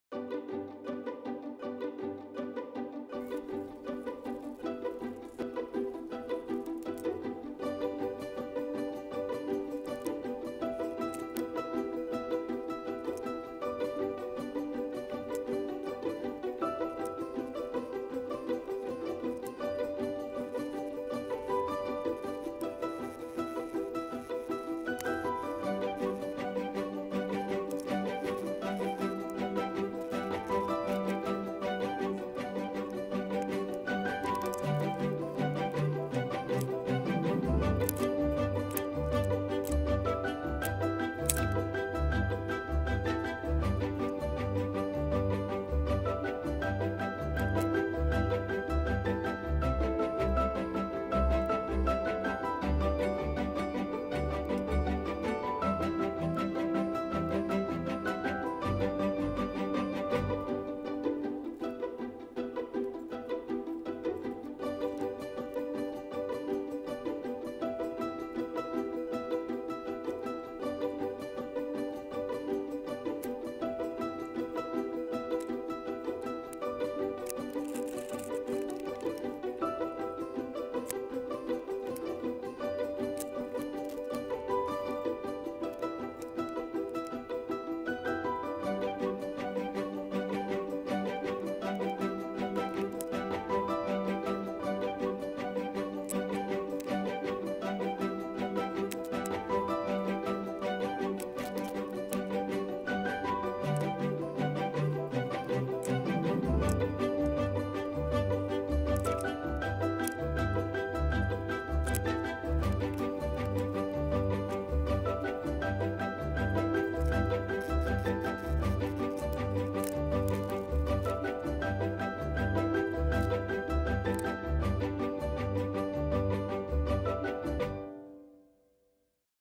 How to paint a Daisy
How to paint a Daisy on a reclaimed cupboard door.